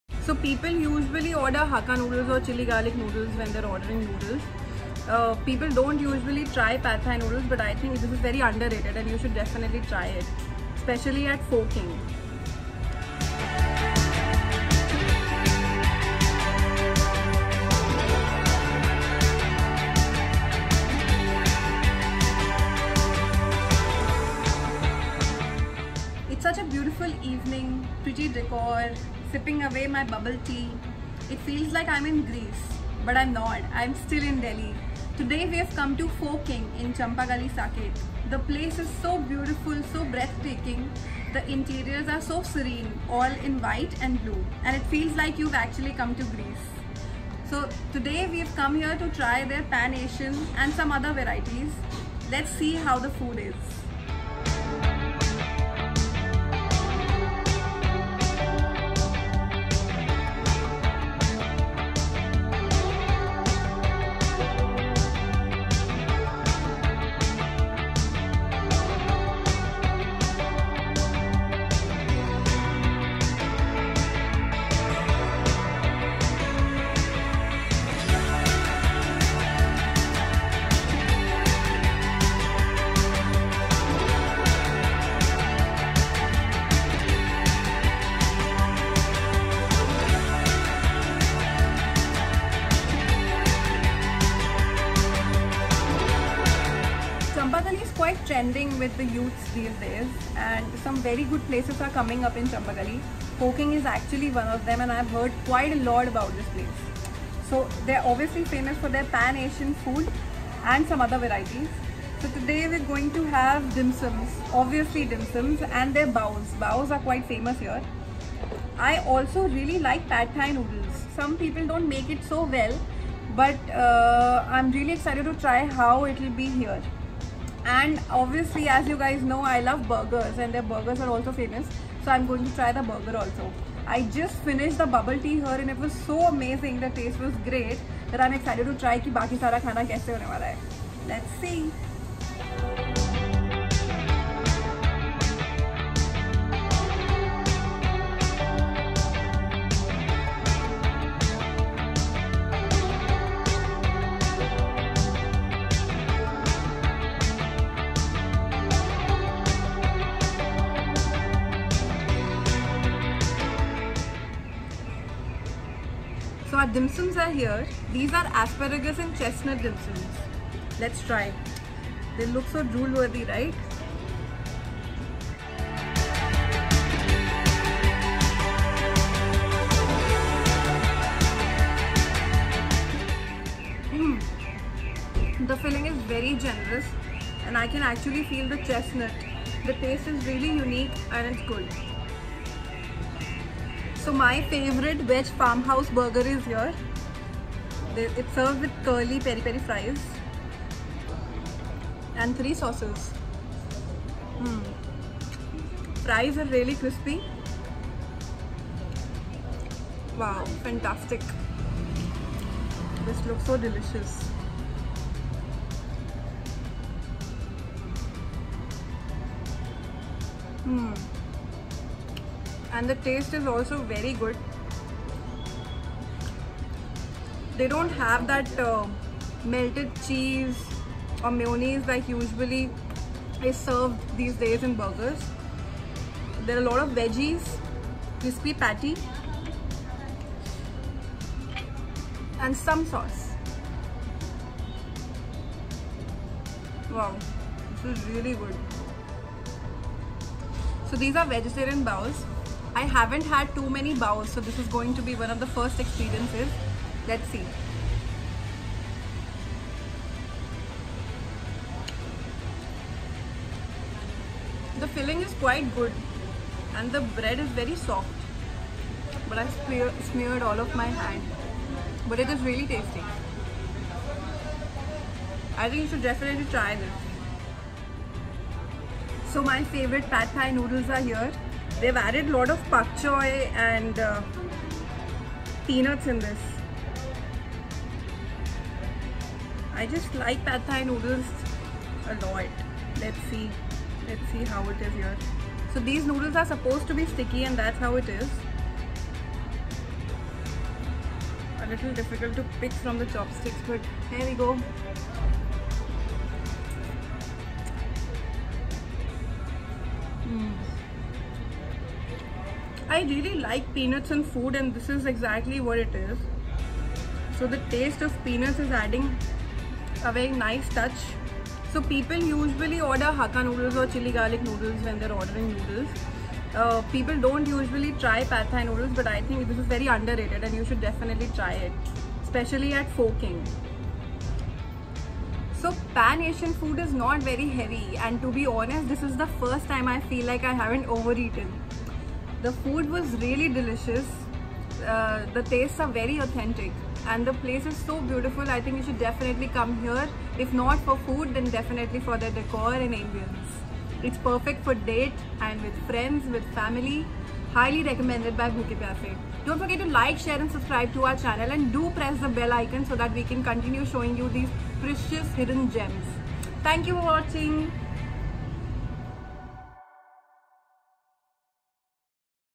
Pho King | Champa Gali | Best Pan Asian | Bubble Tea
So we recently visited Greece in New Delhi. Such a beautiful decor and ambience.

Pan Asian Delicacies, Bubble Tea, Refreshing Mocktails and grills from all around the world.

Watch our video reviewing their food.